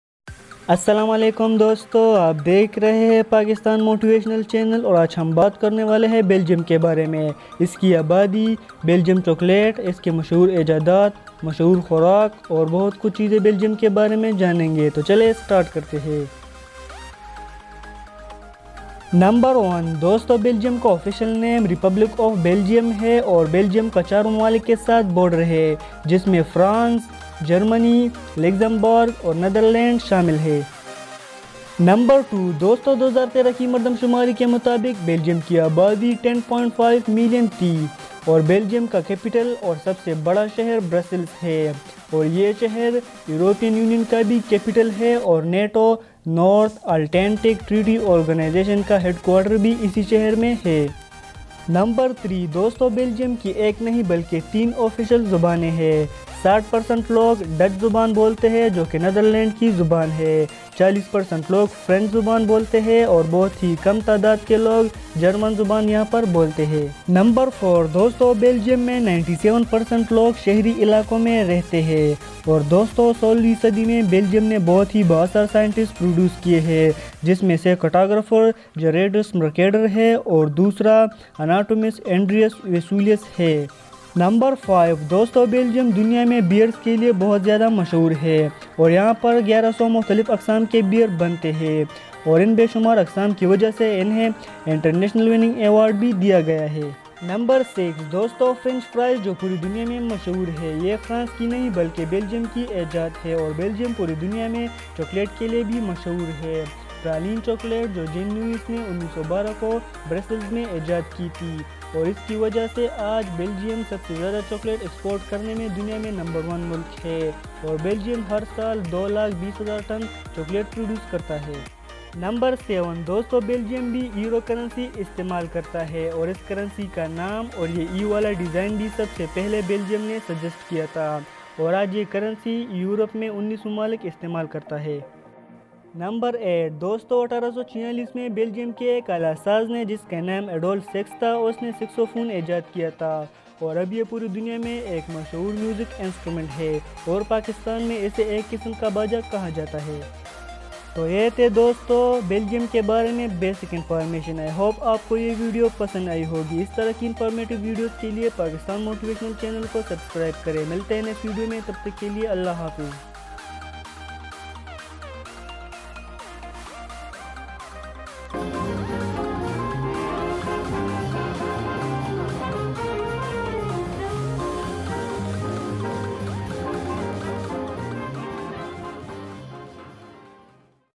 8 Most Vital Facts About Belgium
In This Video I Will Tell You About Belgium Belgium Technology, Economy, Populations, Belgium Chocolate, Beers, Transportation Inventions, Engineering, Companies, Martial arts, Food's National Sport's, Capital And Major Cities and Much More.